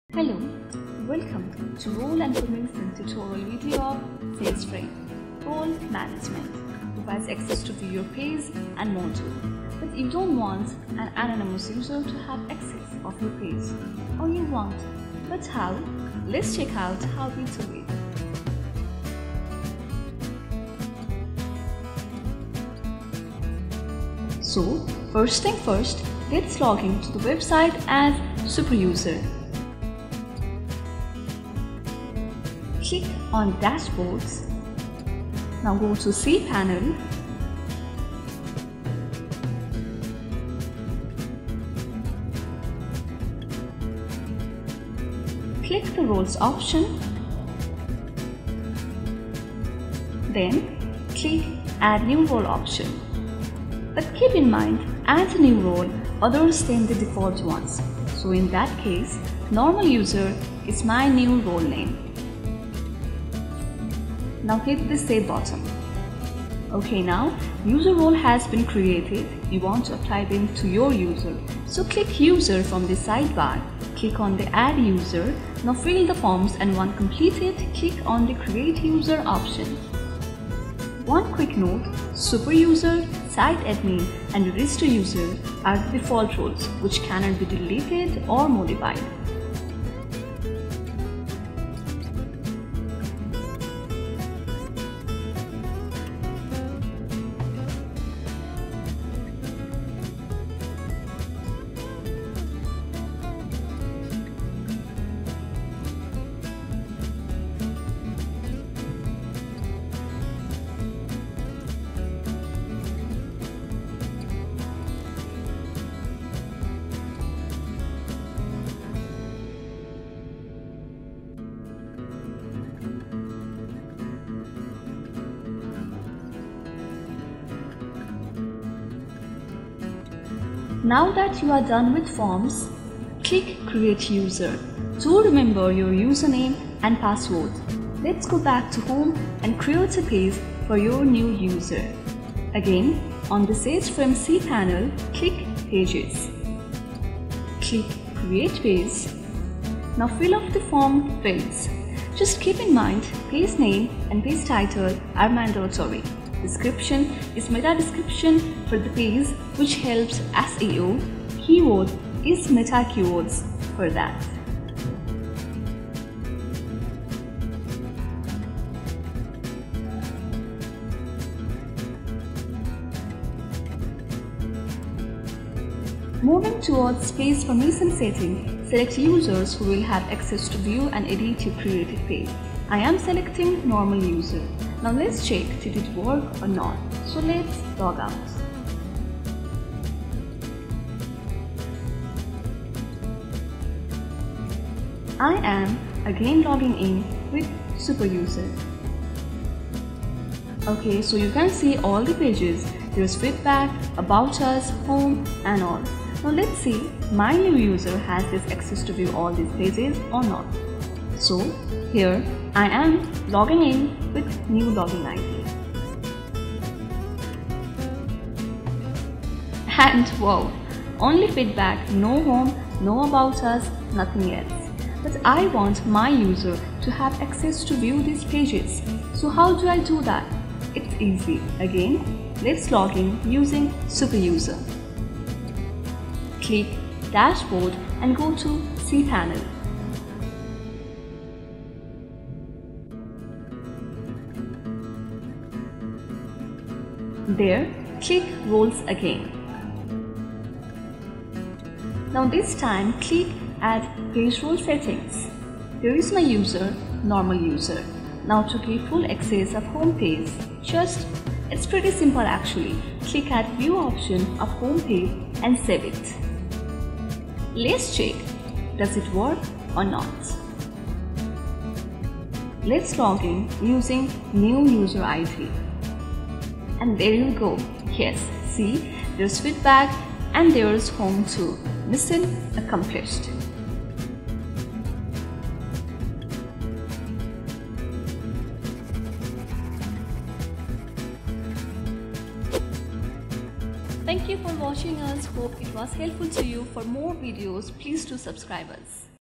Role and Permission/ Access to view
In this Sageframe tutorial we will show you how to configure Users, Roles and Permissions to control administrator to have access to view different areas on a website.